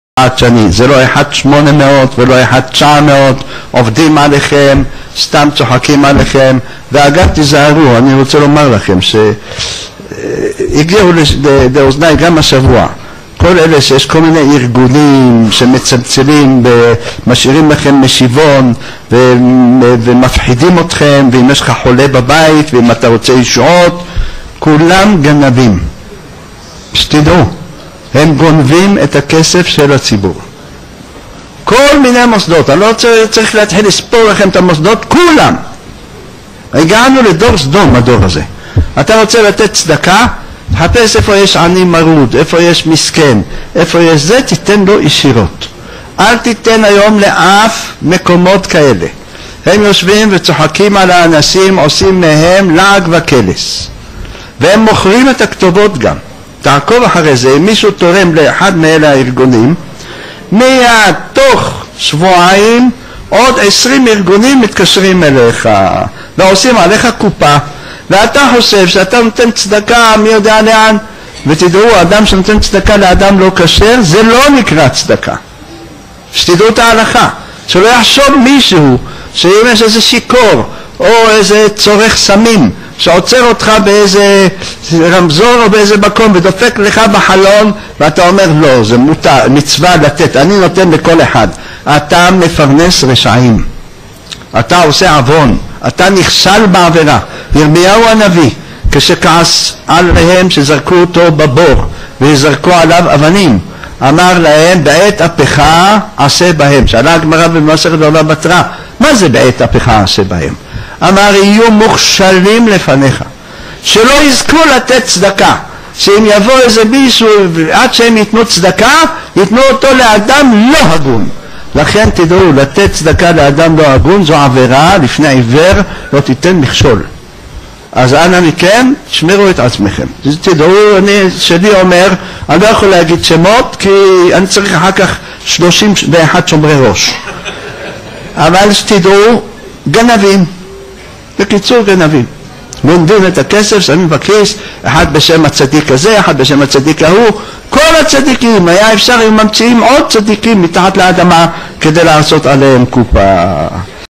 כולם גנבים! רב חביב מגלה למאמינים שיש המון שחיתות בתעשיית התרומות
הרב גם חושש לתת שמות פן ינקמו בו.
ברכות, קמעות וכשרות הן תעשיות שמגלגלות סכומי עתק!

יואלה פרנקו